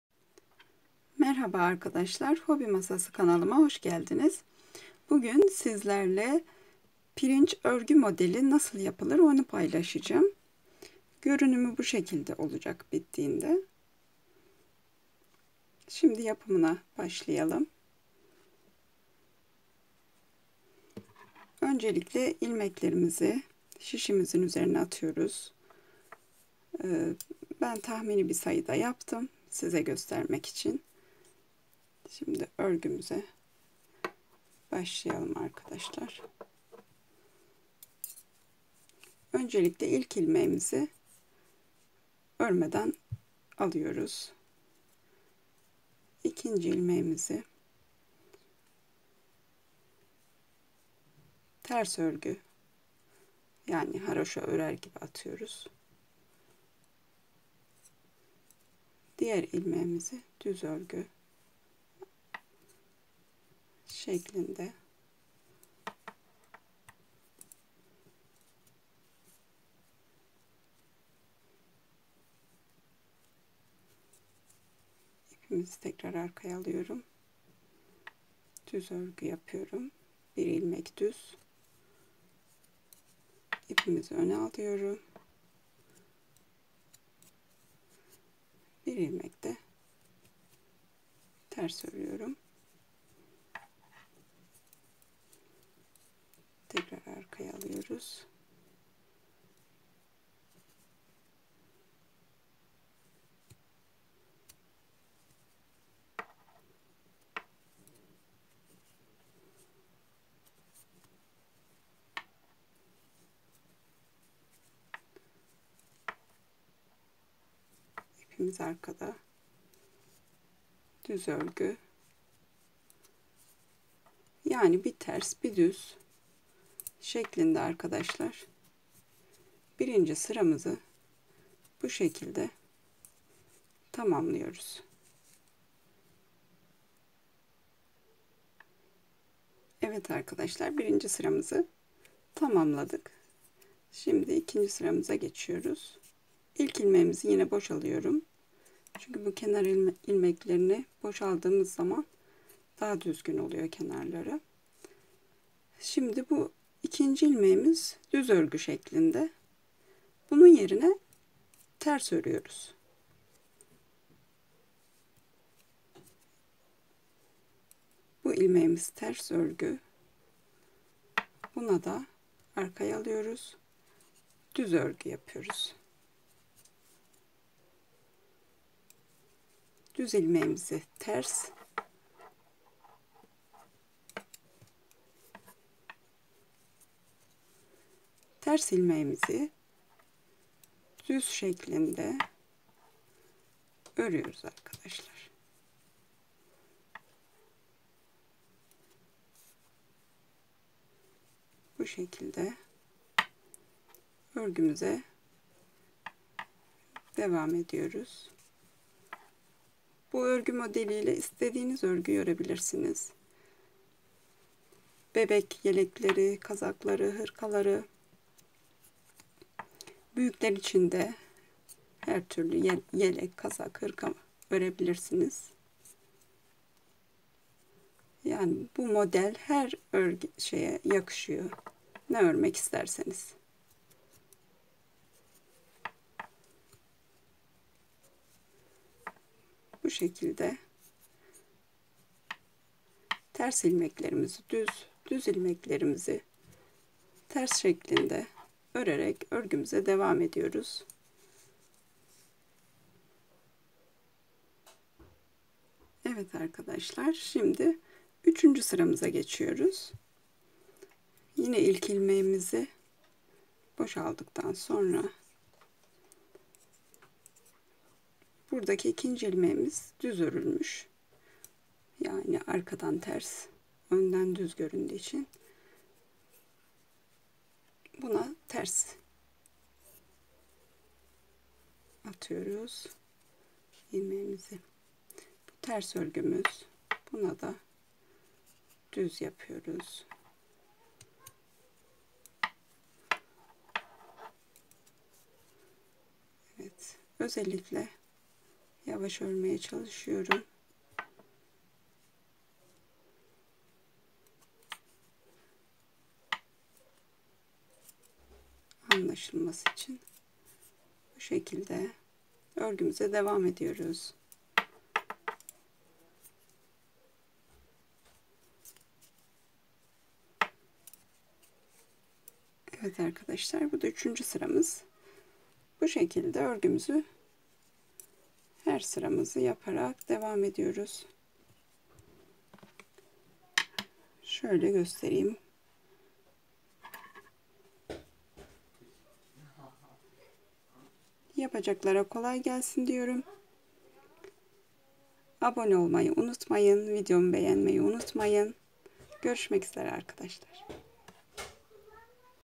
Pirinç örgü modeli nasıl yapılır/Şiş ile pirinç örgü yapımı/Kolay örgü modelleri/kendin yap/DIY
Kanalıma hoşgeldiniz.
Pirinç örgü modeli nasıl yapılır onu paylaştım sizlerle umarım faydalı olur. Sevgiler